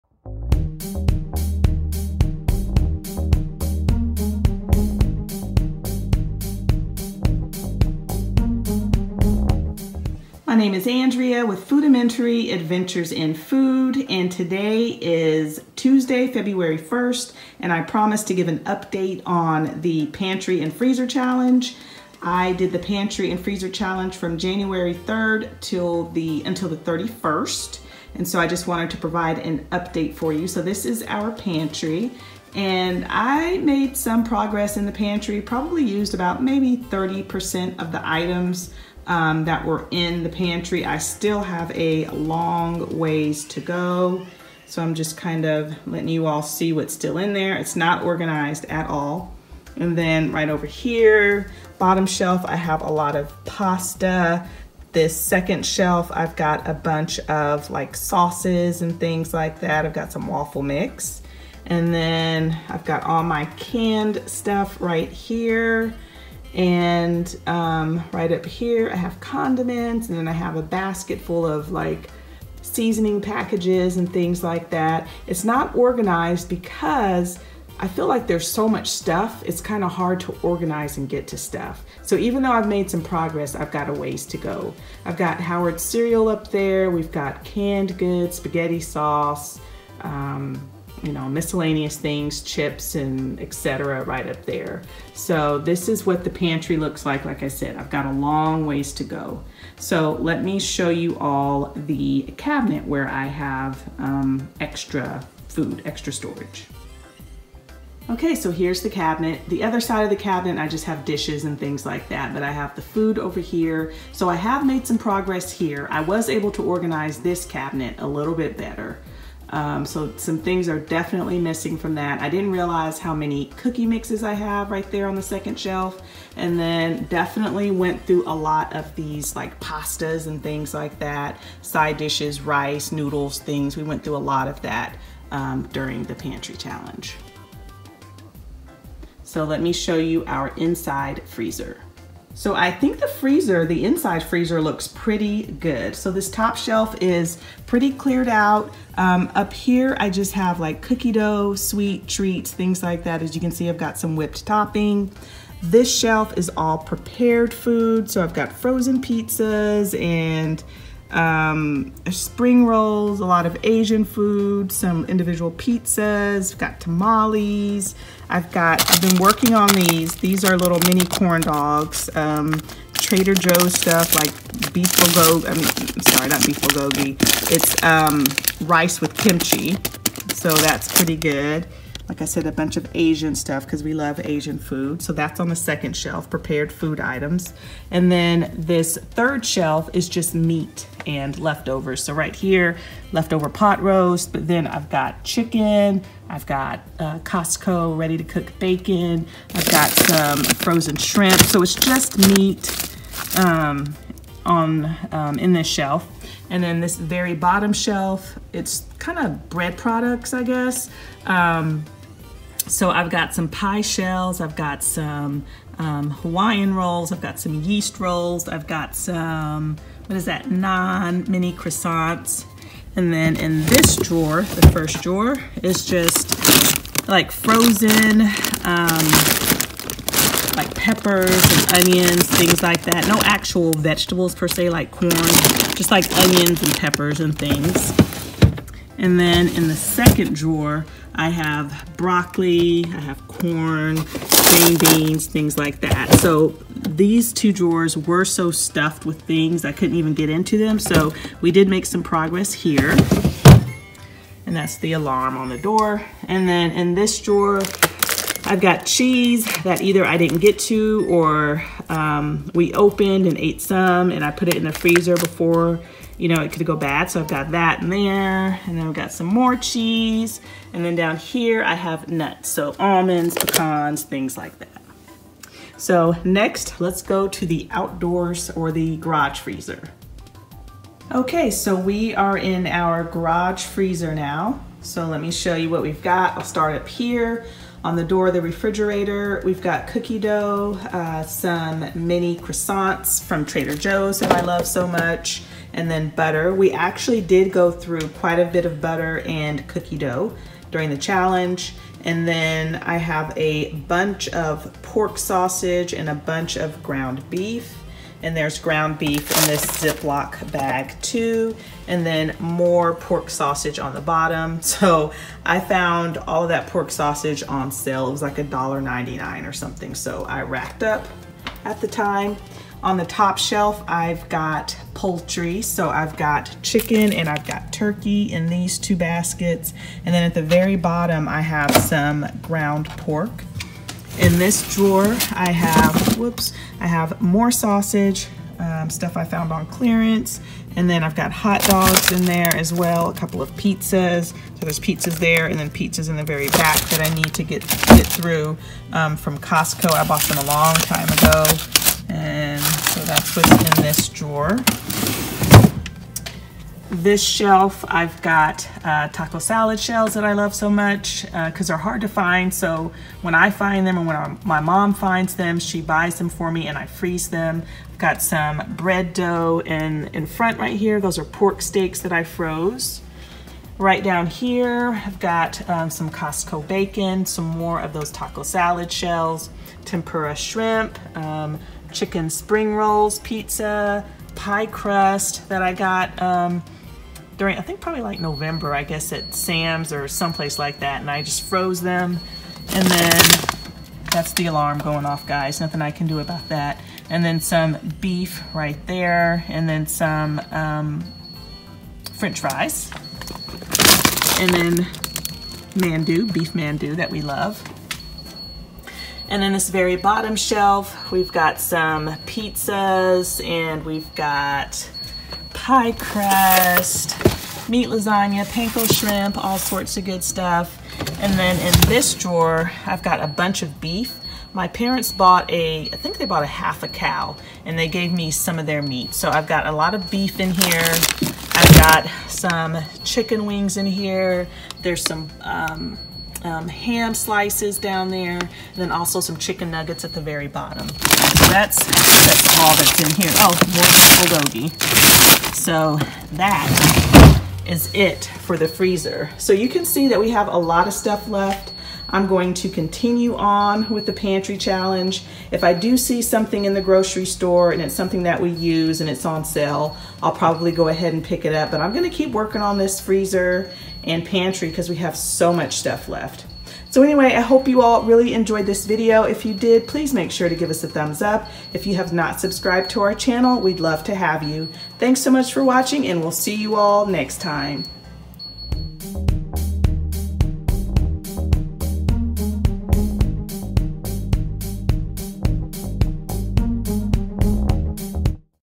What's In My Pantry & Freezer Now? | 30-Day Pantry & Freezer Challenge Update!
Today I'm sharing an update on our pantry and freezer after our 30-day pantry and freezer challenge. If you'd like to see what our pantry and freezer looked like before we started, check out this